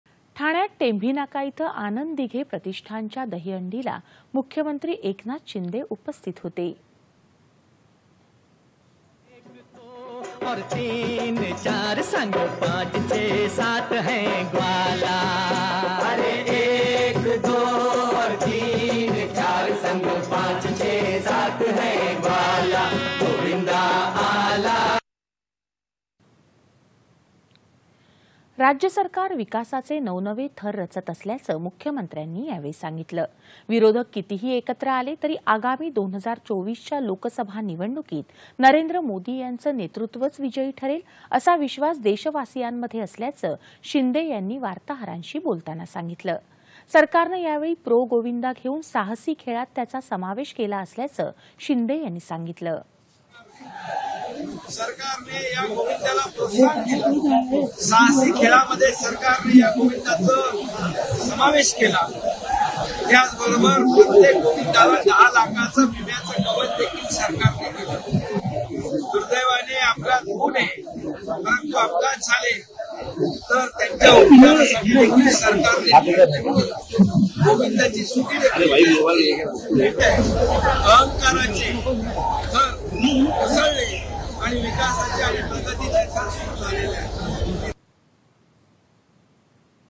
राज्य सरकार विकासाचे नवेनवे थर रचत आहे - मुख्यमंत्री एकनाथ शिंदे
दि. ०७.०९.२०२३ :

   ठाण्यात टेंभी नाका इथं आनंद दिघे प्रतिष्ठानच्या दहीहंडीला मुख्यमंत्री एकनाथ शिंदे उपस्थित होते.
होल्ड .
  राज्य सरकार विकासाचे नवेनवे थर रचत असल्याचं मुख्यमंत्र्यांनी सांगितलं.
 विरोधक कितीही एकत्र आले तरी आगामी २०२४  च्या लोकसभा निवडणुकीत नरेंद्र मोदी यांचं नेतृत्वच विजयी ठरेल, असा विश्वास देशवासियांमध्ये असल्याचं शिंदे यांनी वार्ताहरांशी बोलताना सांगितलं.
 सरकारनं यावेळी प्रो-गोविंदा घेऊन साहसी खेळात त्याचा समावेश केला असल्याचं शिंदे यांनी सांगितलं.
सिंक--- मुख्यमंत्री.

8.30am | 11.00am | 1.00pm | 4.30pm | 7.00pm | 9.30pm